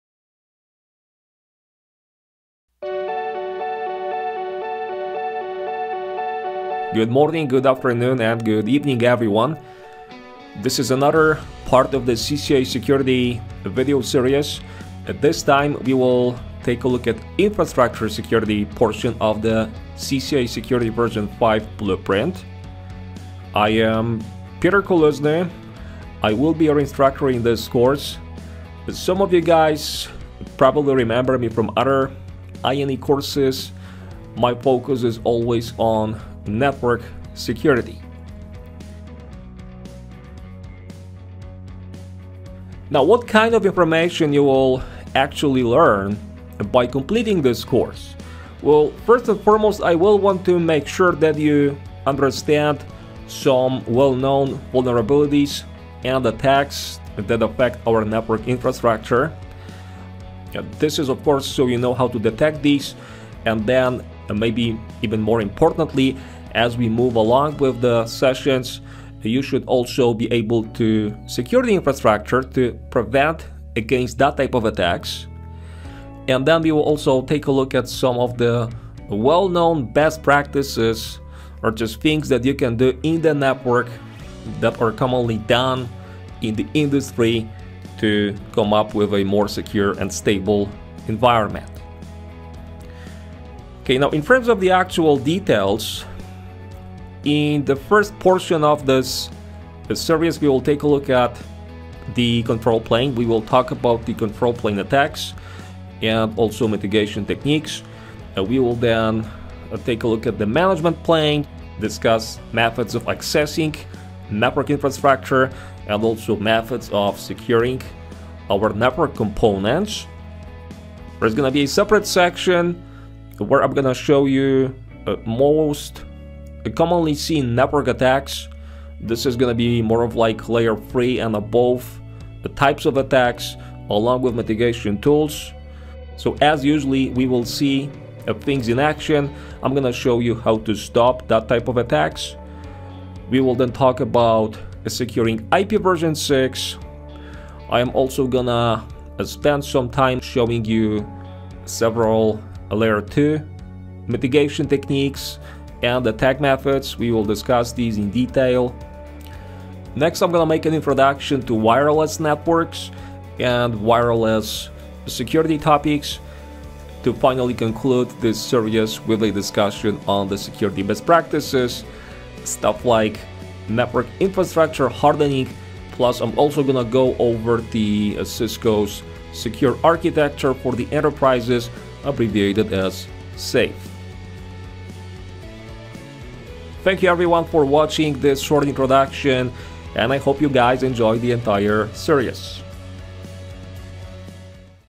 Infrastructure Security
Sign up for an All Access Pass to view this entire course!

This module focuses on the Infrastructure Security part of the current CCIE Security v5 lab & written exam blueprints. It discusses various types of network attacks, mitigation techniques, and other related Control, Management and Data Plane security features. By completing this course, you will understand how to detect and mitigate common network attacks, such as IP Spoofing, DoS, DDoS, IP Fragmentation/Options/Extension Header attacks, Layer 2 attacks, IPv6 attacks, and more. This course will also teach you how to configure and verify certain IPv4 and IPv6 infrastructure security technologies, such as Control Plane Policing and Protection, Management Plane Protection, Secure Neighbor Discovery and Wireless Security.